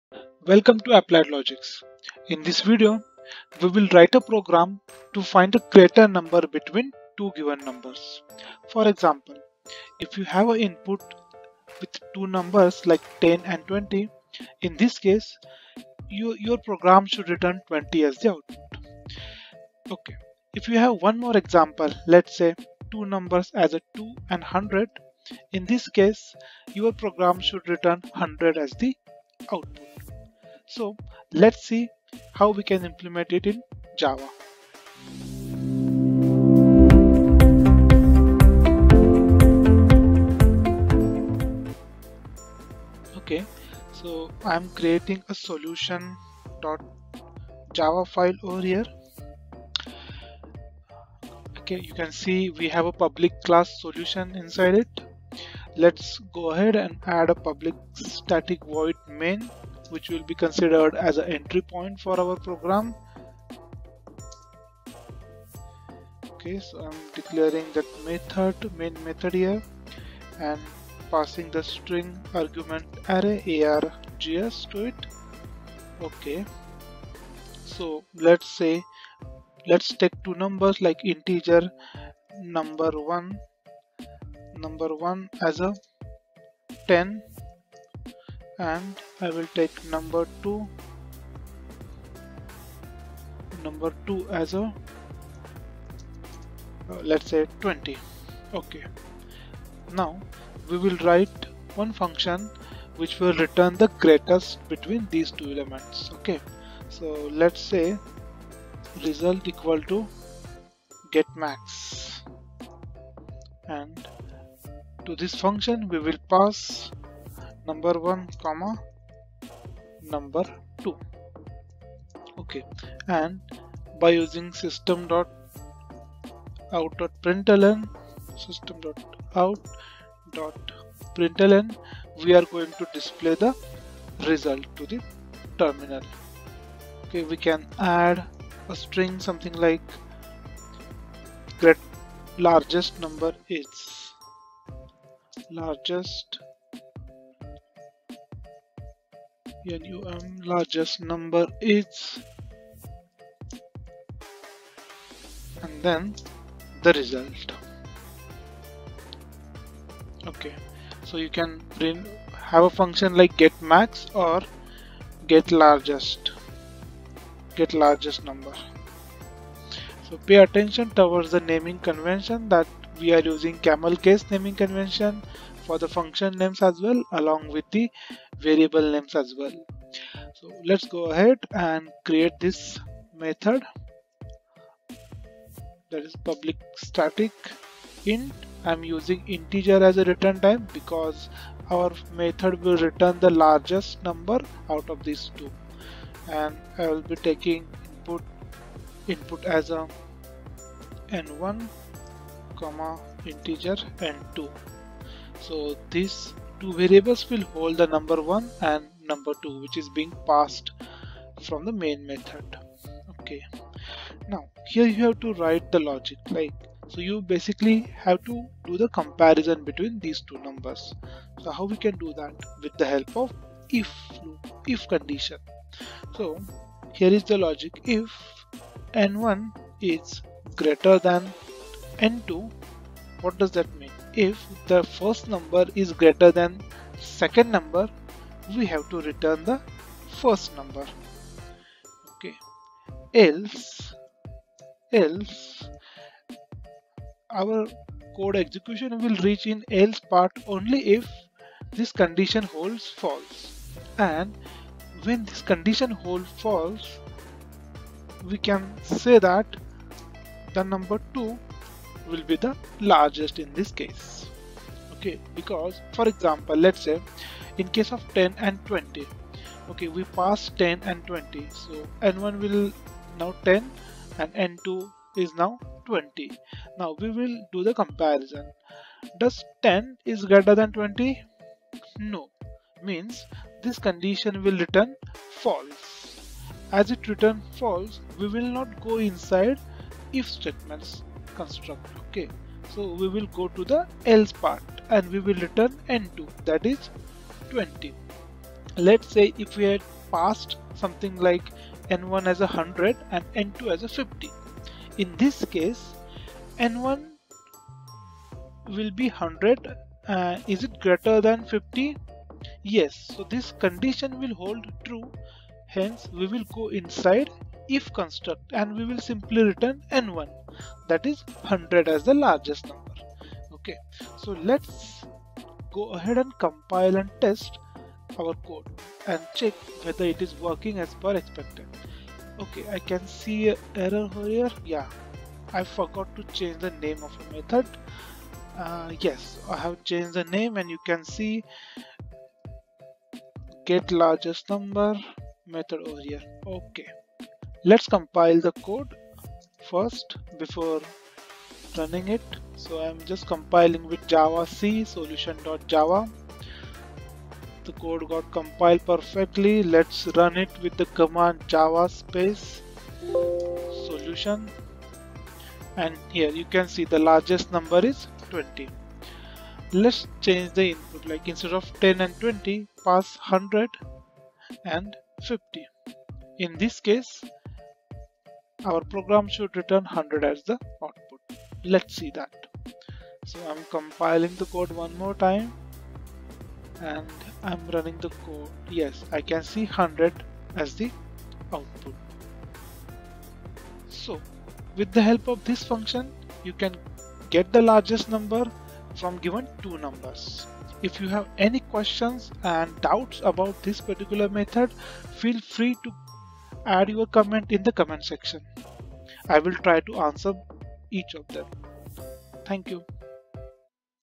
Java program to find largest of two numbers | Programming For Beginners
In this video, we will write a java program to find the largest of two numbers.